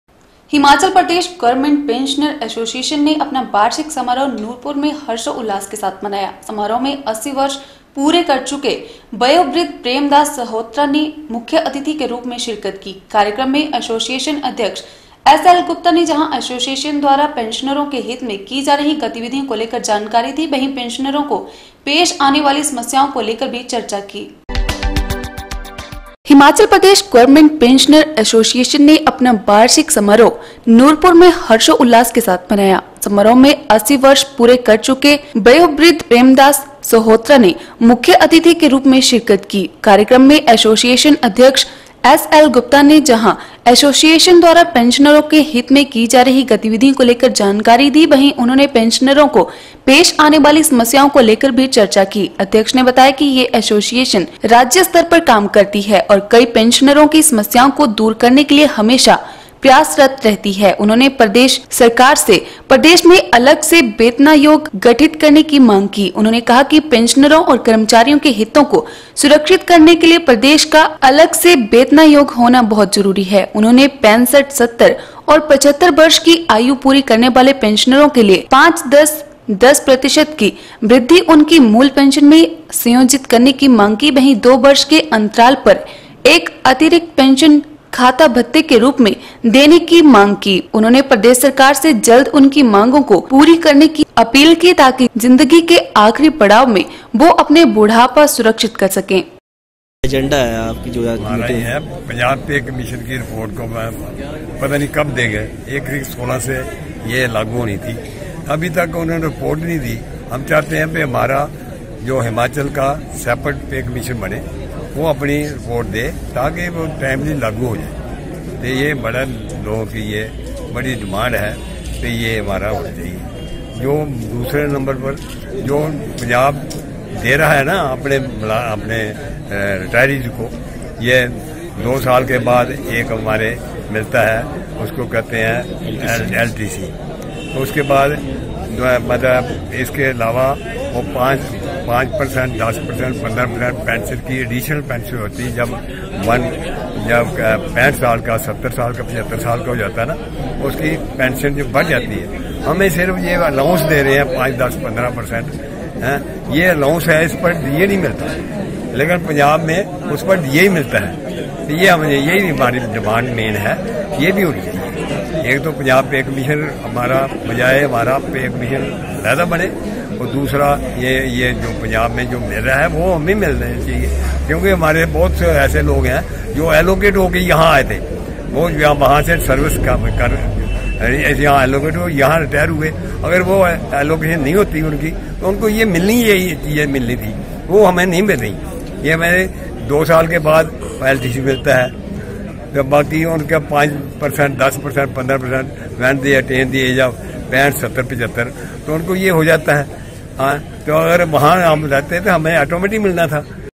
14 JAN N9 hp गवर्नमेंट पेंशनर एसोसिएशन ने अपना वार्षिक समारोह नूरपुर में हर्षोल्लास के साथ मनाया।
हिमाचल प्रदेश गवर्नमेंट पेंशनर एसोसिएशन ने अपना वार्षिक समारोह नूरपुर में हर्षोल्लास के साथ मनाया।समारोह में अस्सी वर्ष पूरे कर चुके वयोवृद्ध प्रेमदास सहोत्रा ने मुख्यतिथि के रूप में शिरकत की।कार्यक्रम में एसोसिएशन अध्यक्ष एसएल गुप्ता ने जहां एसोसिएशन द्वारा पेंशनरों के हित में की जा रही गतिविधियों को लेकर जानकारी दी वहीं उन्होंने पेंशनरों को पेश आने वाली समस्याओं को लेकर भी चर्चा की